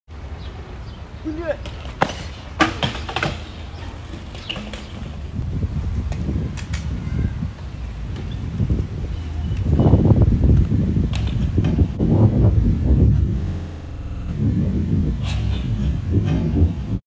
Плаза Краснодар ☀️
Один трюк с Краснодара, оставлю на память, плазу протер, людей не убил 🙏